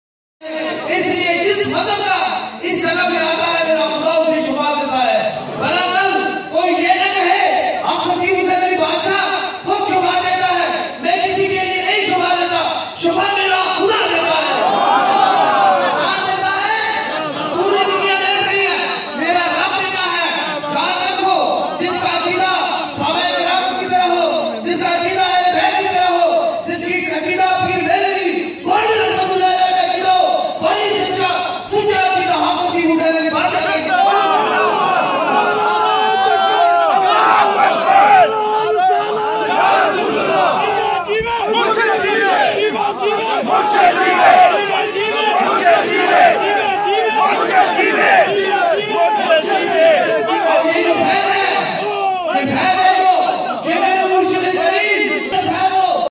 22nd Annual Mehfil e Naat Manchester Sahabzada Haq Khateeb Hussain Ali Badshah part 32
Thousands Attended 22nd Annual Mehfil e Naat Manchester. Sahabzada Haq Khateeb Hussain Ali Badshah Attended as a Chief Guest from Pakistan.